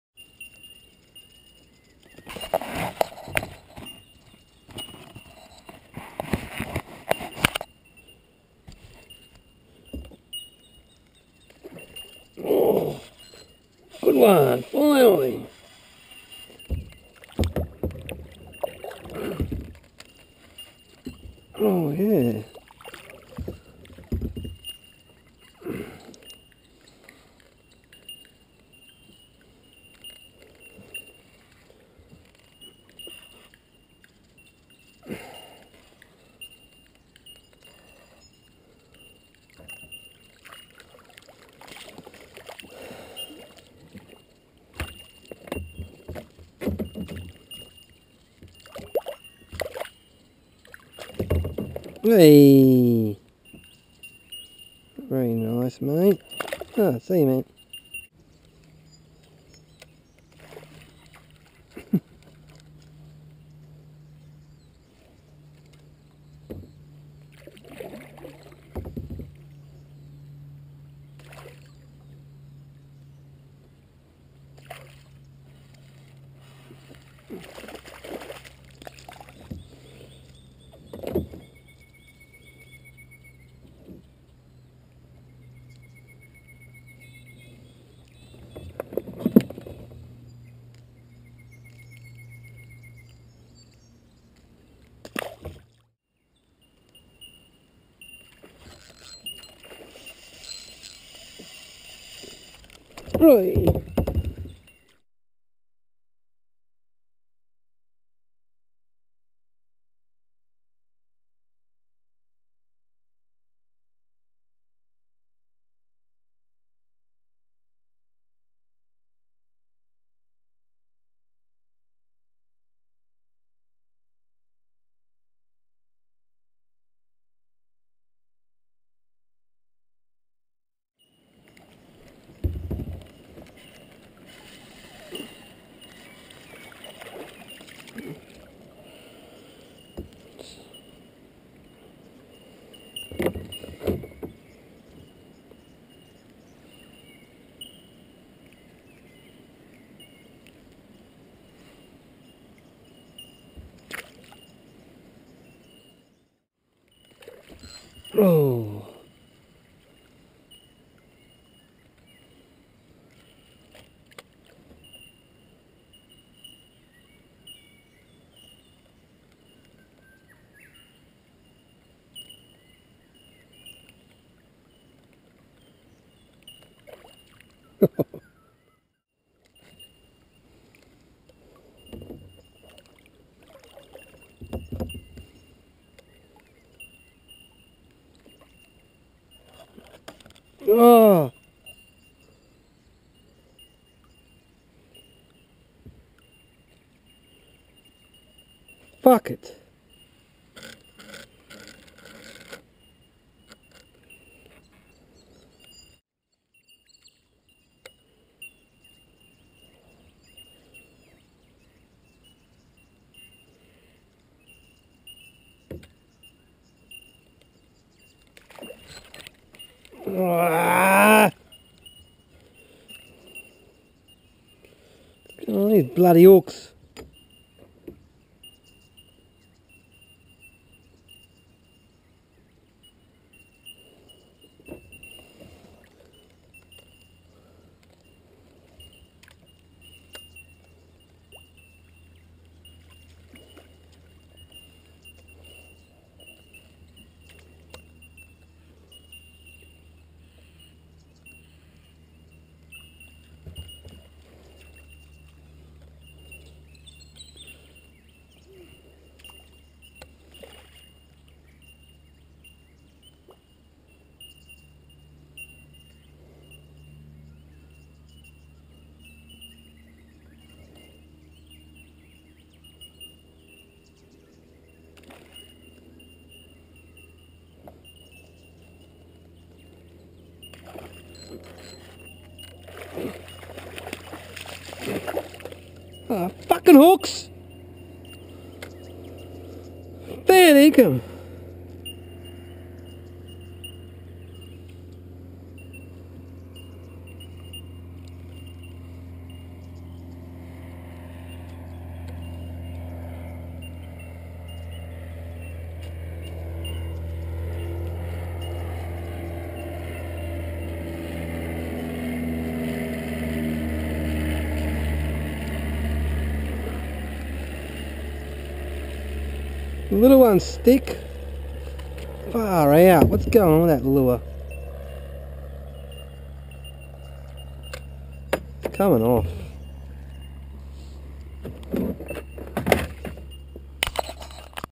A few Bass
Got a few small fish,missed A lot of nice ones , Lost a Mega Bass Siglett.
Sorry about the Swearing Cheers